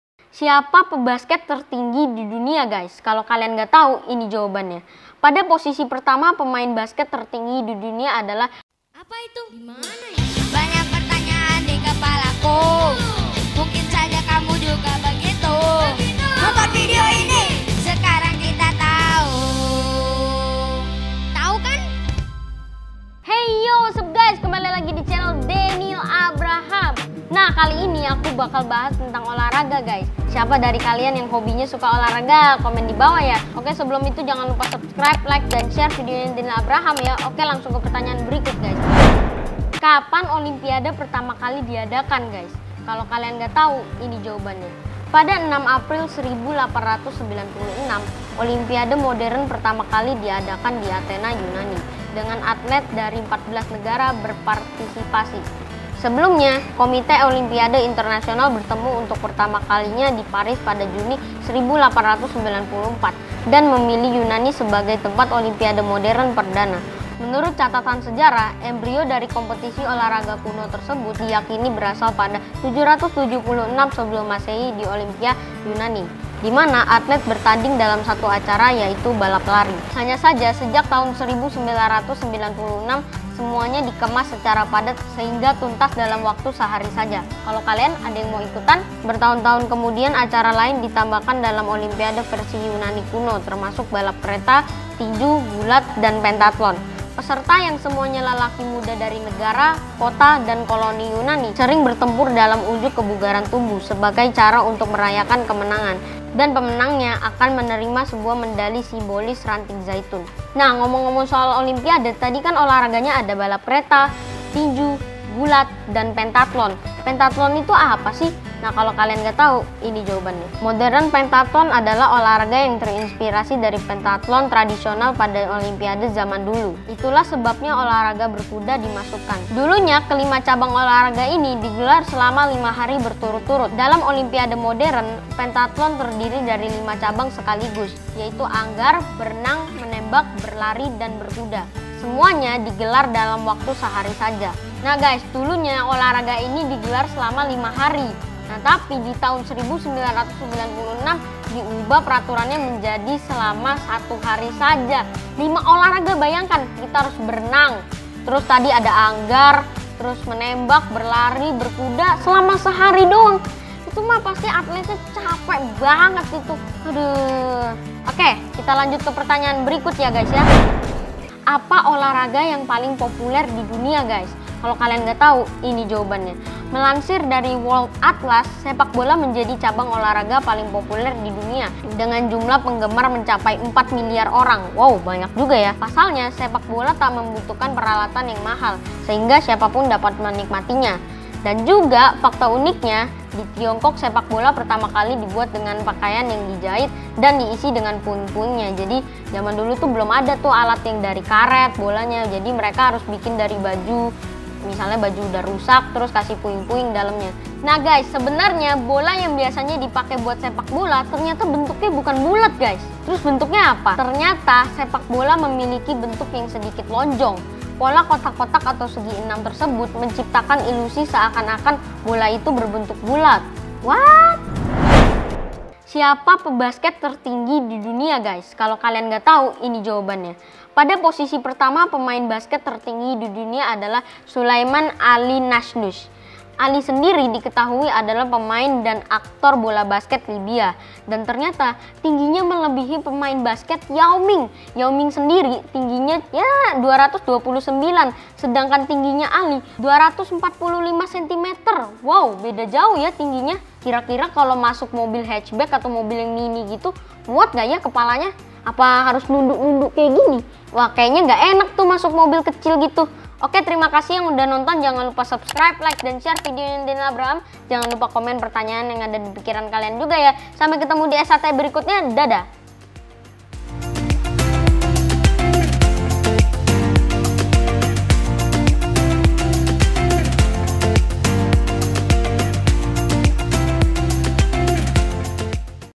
SEKARANG AKU TAHU - SEPAK BOLA (EPS 21)
Daniel Abraham Santoso (DAS) pada konten "Sekarang Aku Tahu" akan membahas pertanyaan-pertanyaan yang ada dipikiranku dipikiran kalian.

Di Video SAT kali ini akan membahas hal-hal tentang OLAHRAGA